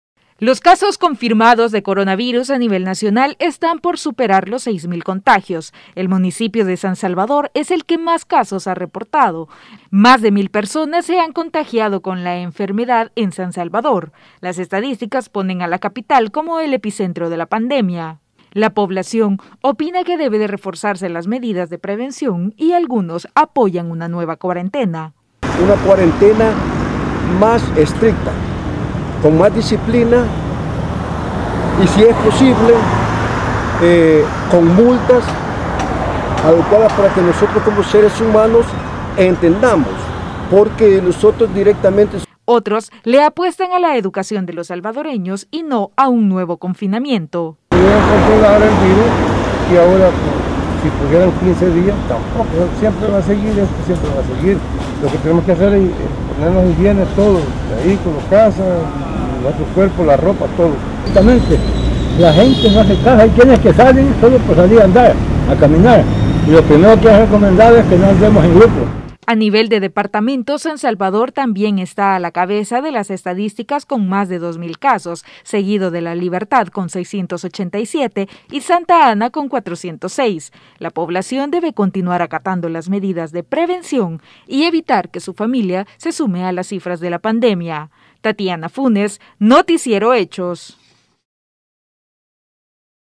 San Salvador, epicentro de la pandemia por covid 19 en El Salvador
El departamento de San Salvador, capital de El salvador, es el epicentro de la pandemia por el covid-19 luego de presentar más de los 2 mil casos positivos.

¡Visítenos!